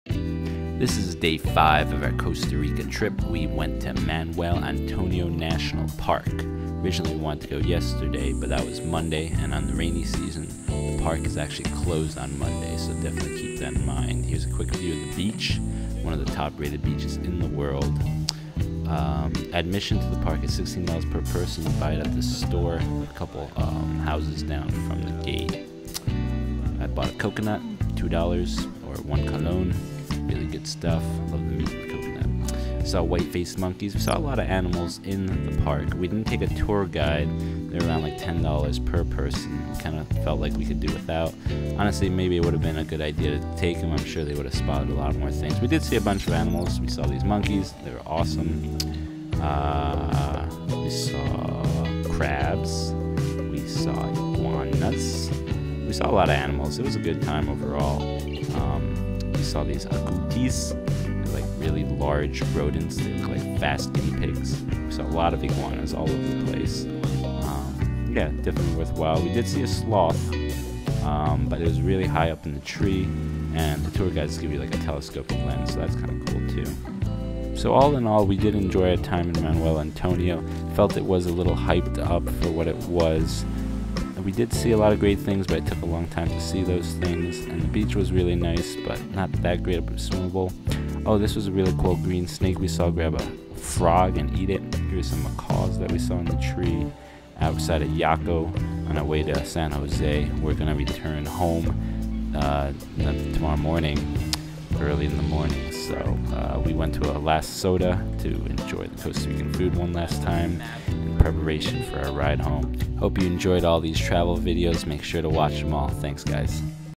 Manuel Antonio National Park - Costa Rica Day 6
Day 6 of our Costa Rica trip visiting Manuel Antonio National Park.

$16USD to enter the park. No re-entry allowed. Closed on Monday’s during the Rainy Season.

Recommend getting a Tour Guide ($10 per person)

Animals we saw:
-Sloths(3)
-White Faced Monkeys (15)
-Green Snake (1)
-Tucans (2)
-Land Crabs (Lots)
-Iguanas (4)